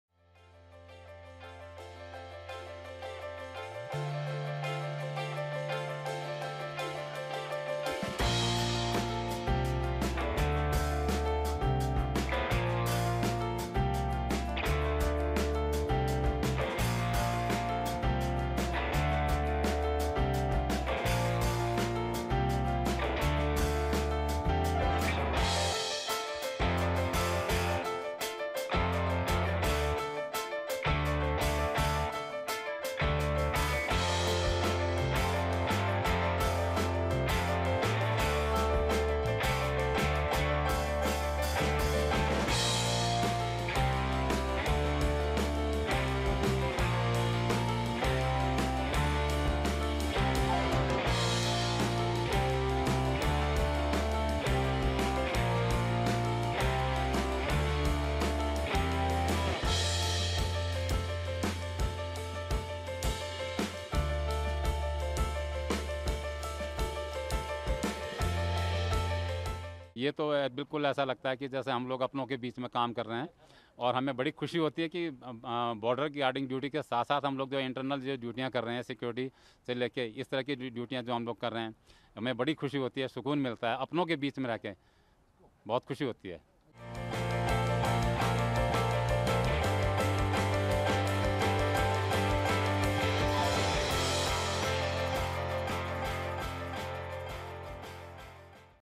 ITBP distributes masks, sanitisers, spray pumps to Udhampur villagers
Udhampur (J&K), Apr 04 (ANI): Indo-Tibetan Border Police organised a COVID-19 awareness camp in far-flung and impoverished villages located in Jammu and Kashmir’s Udhampur. It was ITBP’s 8th camp in the district aimed to educate locals about the pandemic and the required precautions. ITBP officials distributed masks, sanitisers, spray pumps and soaps to the locals. One ITBP official said, “We enjoy doing this work and it feels like we are working among our people. It makes us feel happy.”(NK)